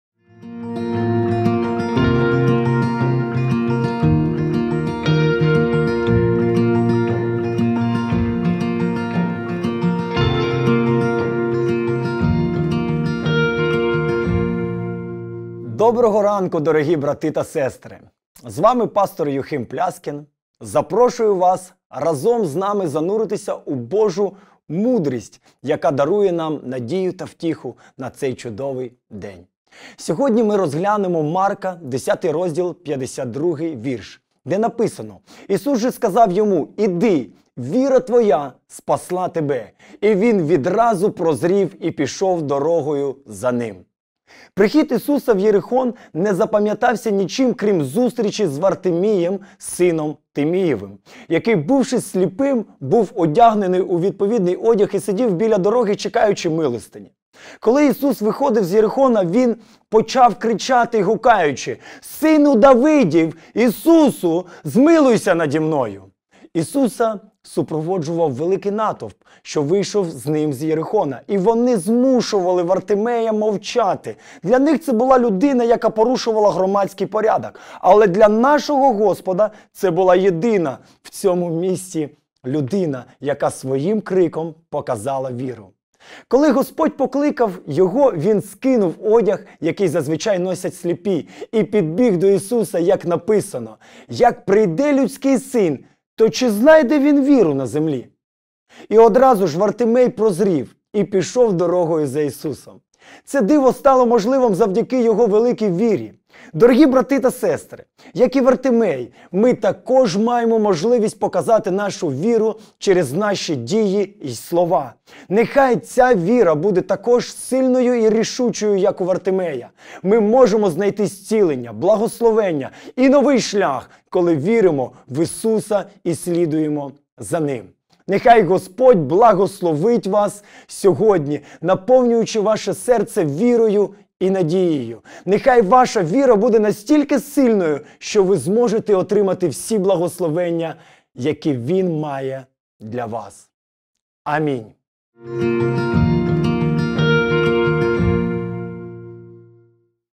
«Щоденний хліб» / 25 серпня / Юхим Пляскін
«Ісус же сказав йому: Іди, віра твоя спасла тебе! І він відразу прозрів і пішов дорогою за Ним» Марка 10:52 (UTT)